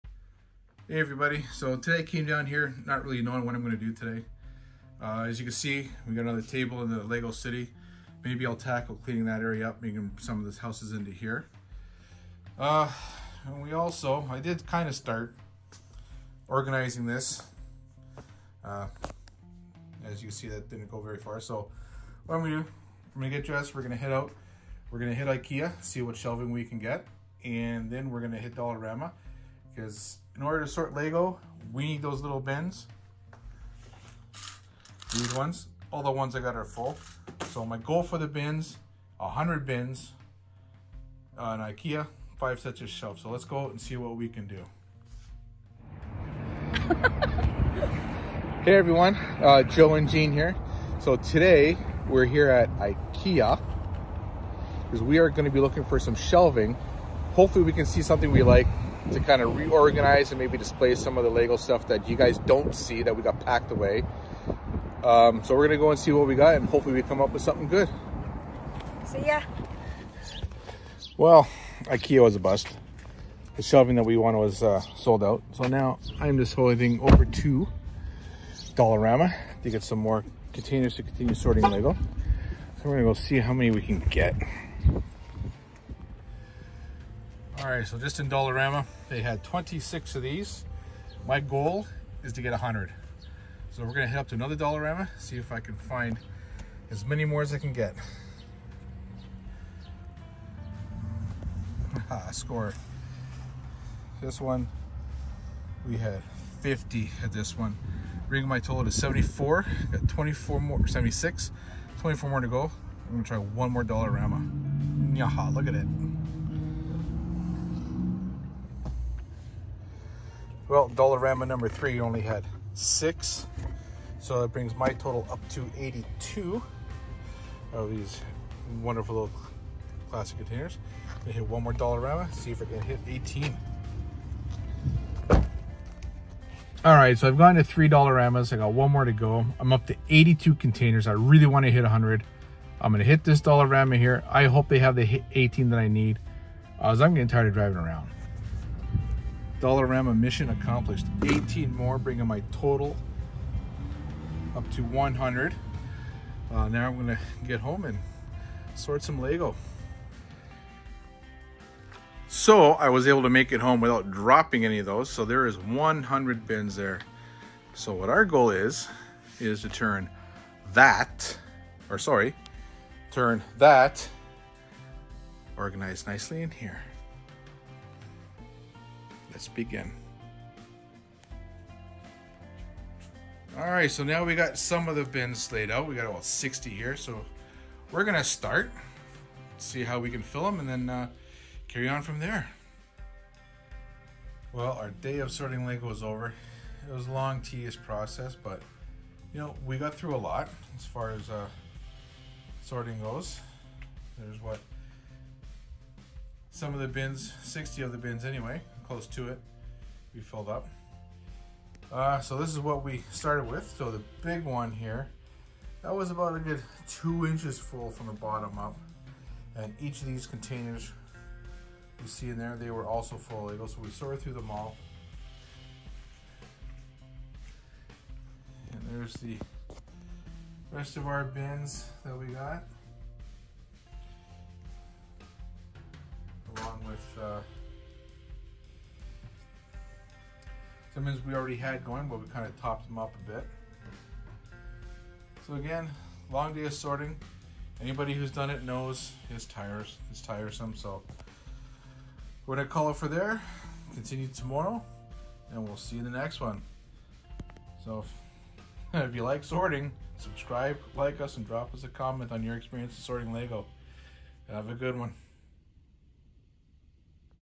What a Fun Time…sorting Lego!
Decided to sort our loose Lego. So I went out looking for shelving and 100 plastic bins!